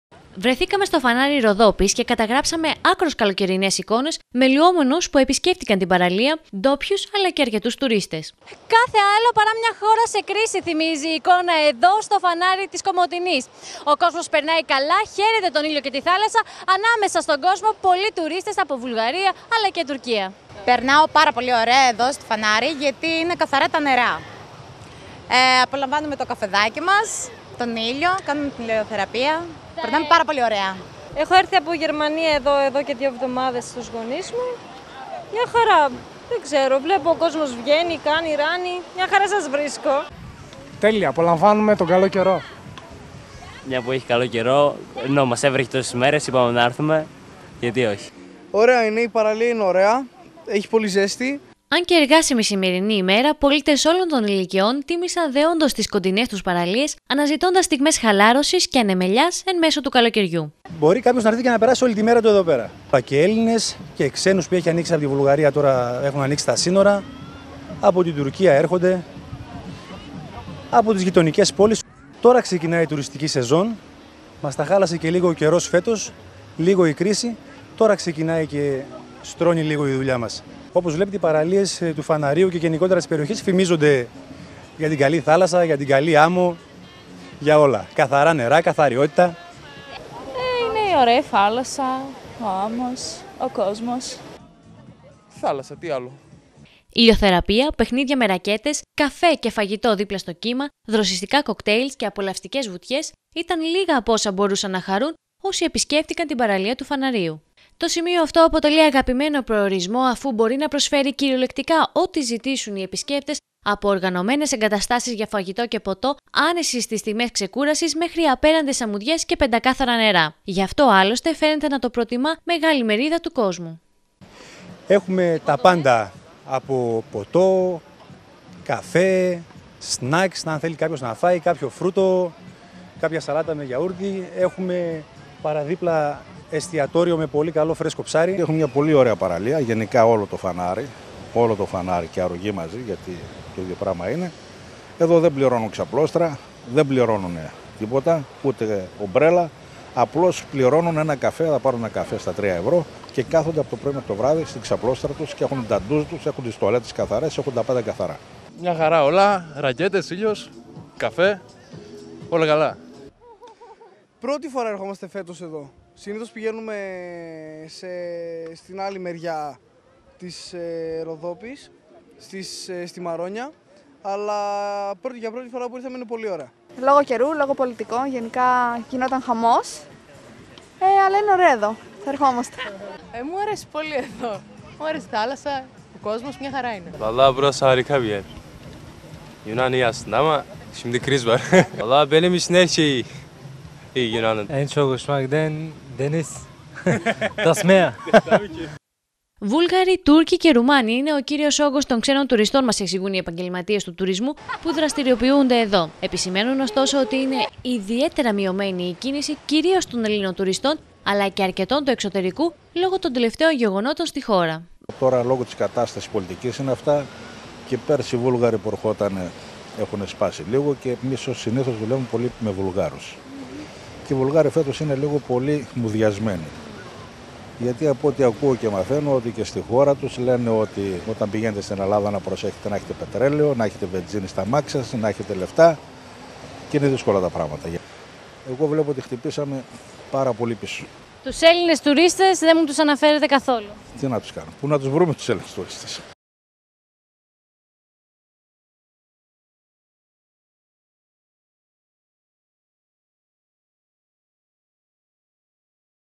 ΕΝΤΟΝΗ Η ΠΑΡΟΥΣΙΑ ΤΩΝ ΛΟΥΟΜΕΝΩΝ ΣΤΙΣ ΠΑΡΑΛΙΕΣ ΦΑΝΑΡΙΟΥ
Στις παραλίες του Φαναριού Ροδόπης βρέθηκε η Άλφα τηλεόραση και κατέγραψε άκρως δροσιστικές εικόνες με παραθεριστές να απολαμβάνουν τον ήλιο, την θάλασσα και τις παροχές των ντόπιων τουριστικών επιχειρήσεων.
Οι επισκέπτες ζούσαν στιγμές καλοκαιρινής ξεγνοιασιάς, ενώ την ίδια ώρα εν μέσω των οικονομικών δυσχερειών οι επιχειρηματίες καρδιοχτυπούσαν για την μειωμένη σε σχέση με πέρυσι, προσέλευση των τουριστών.